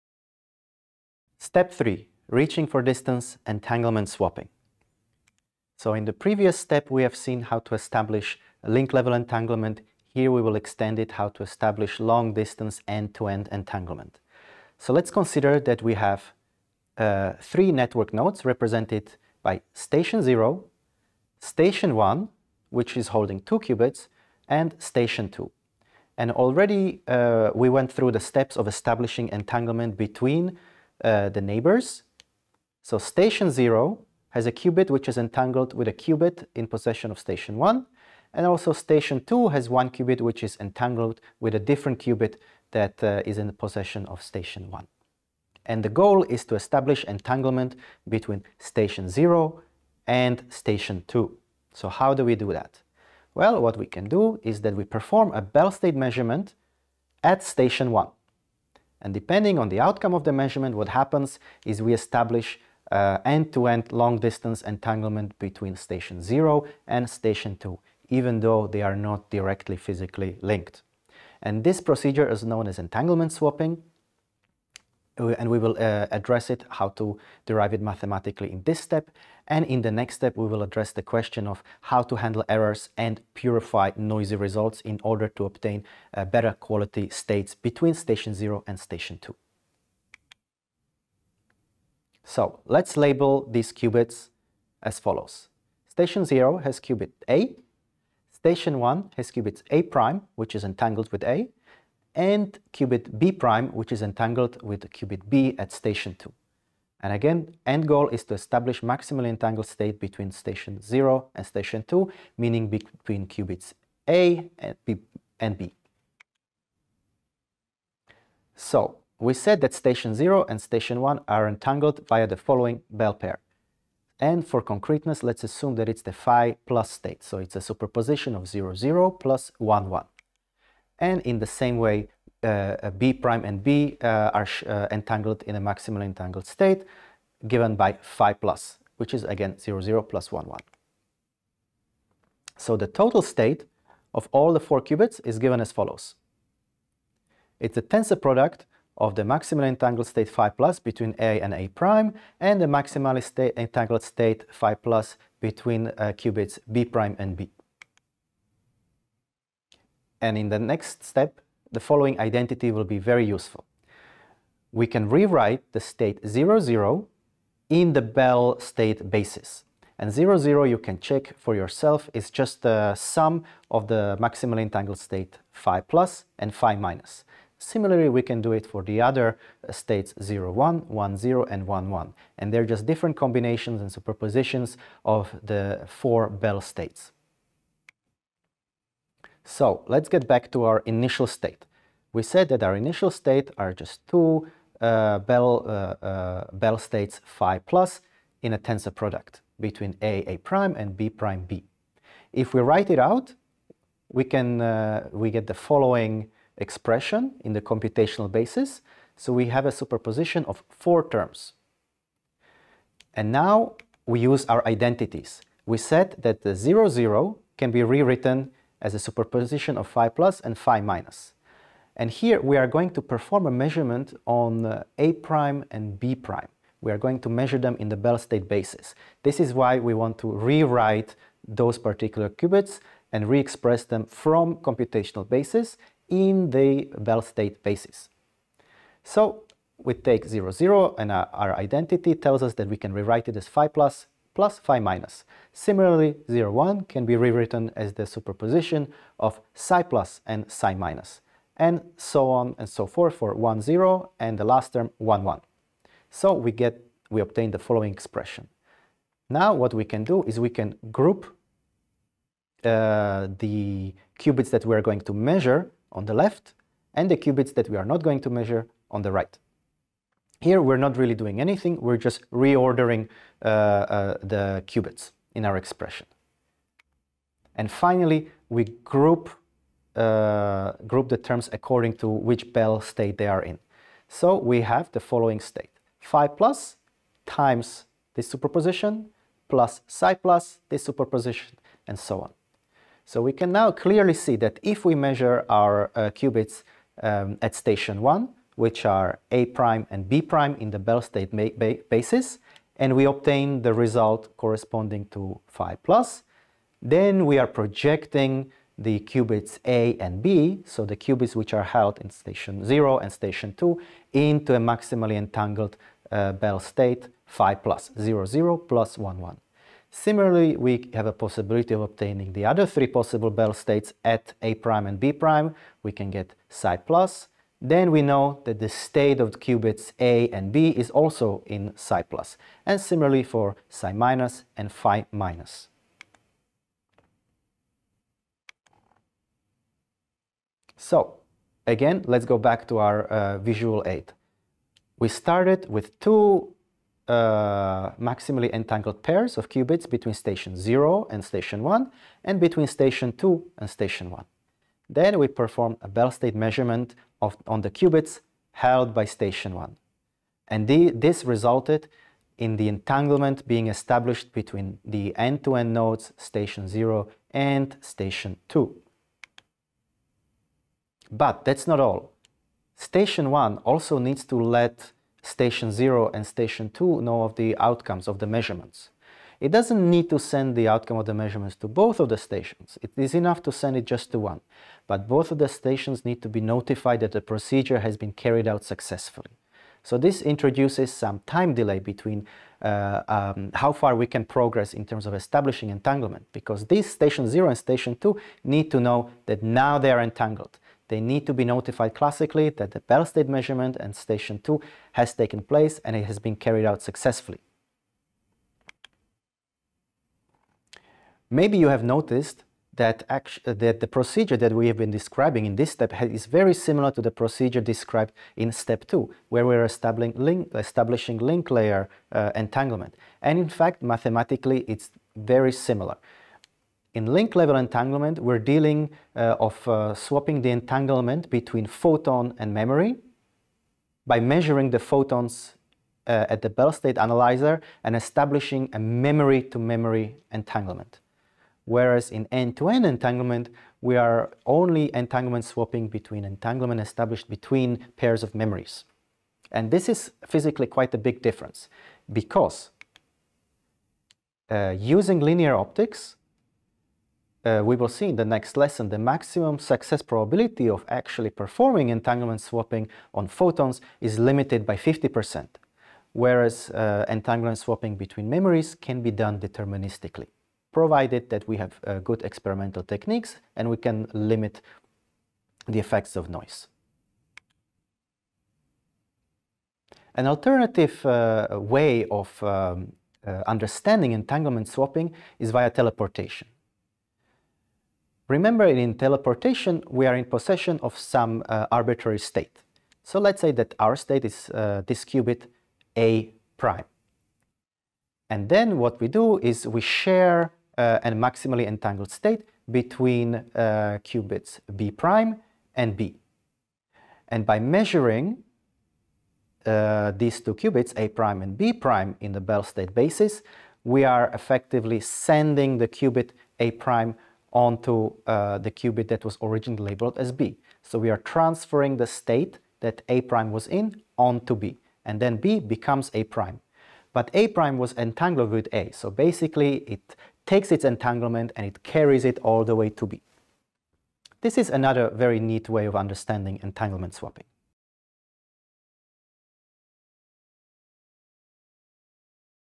12-3 Reaching for distance: Entanglement swapping
Lesson 12 Quantum repeaters
Step 3: Reaching for distance: Entanglement swapping
Having learnt how link-level entanglement can be achieved, we need a way to extend it to further nodes. This is done by entanglement swapping, one of the fundamental primitives in quantum networking.

This is part of our module: "Overview of Quantum Communications".